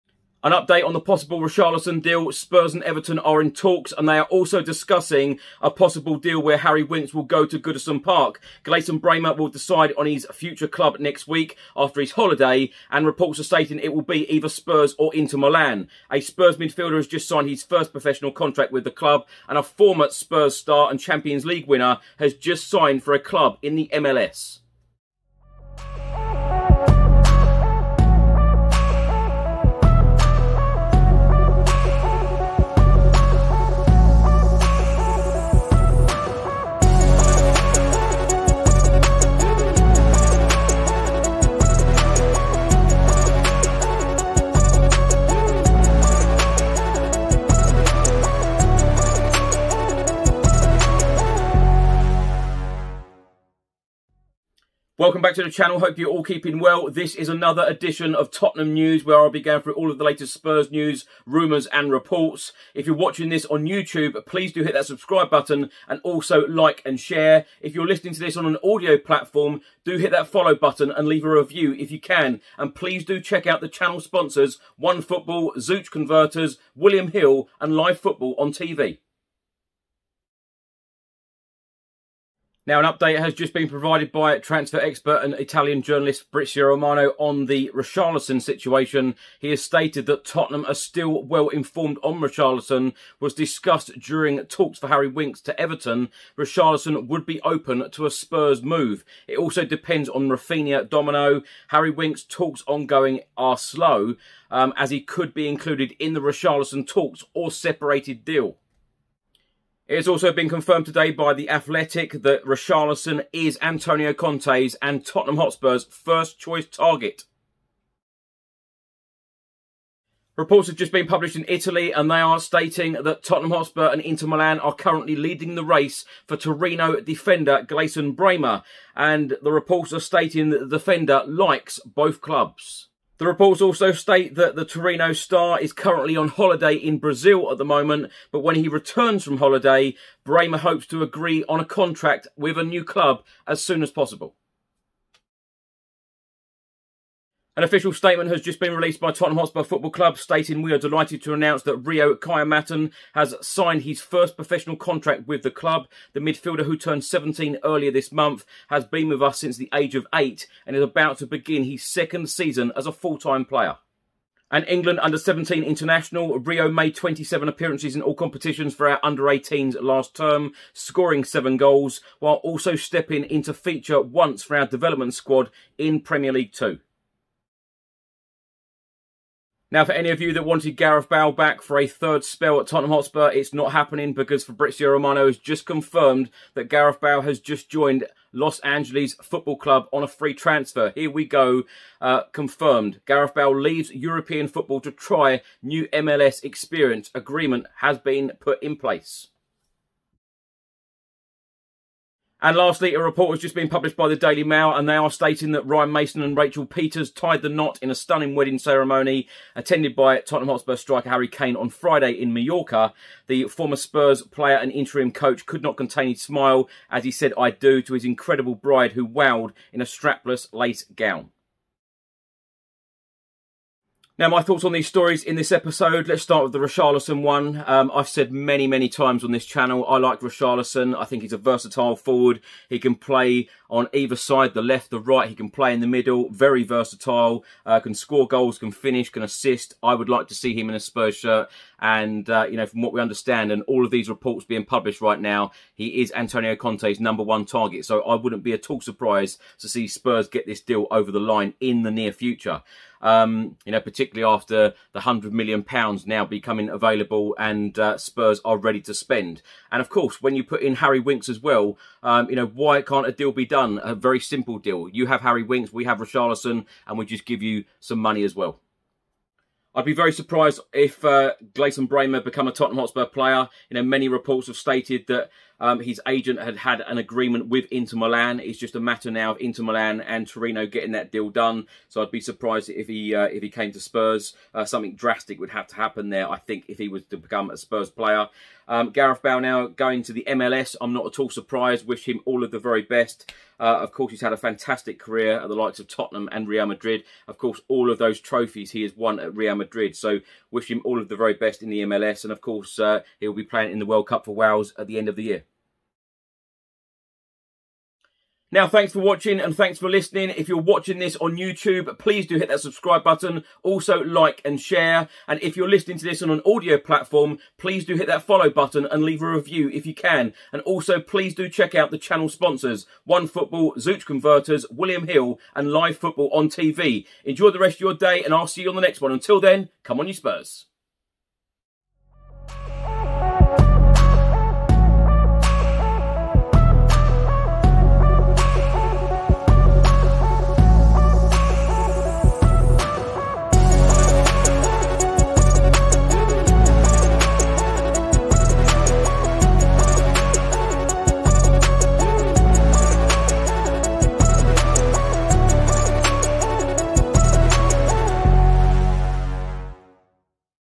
TOTTENHAM NEWS: Richarlison Update, Winks to Everton, Bremer Decision, Kyerematen's Contract, Bale
+ CONTENT:
* Chris Cowlin talks about all of the latest Tottenham news, rumours and reports